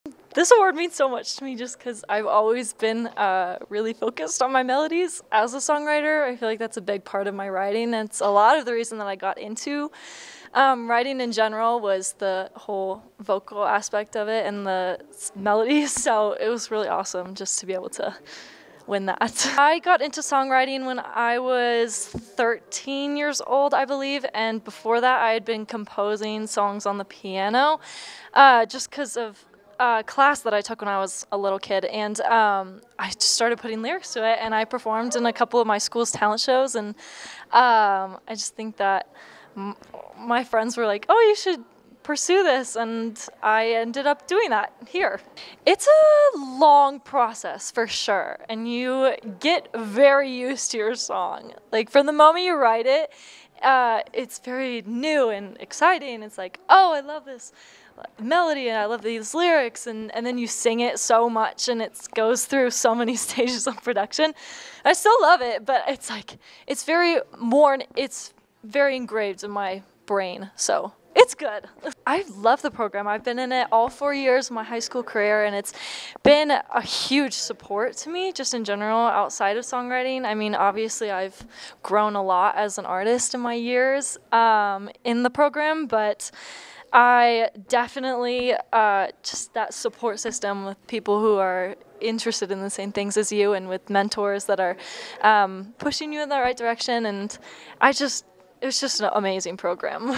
Abby Kloepf
Abby had a successful win of the MELODY award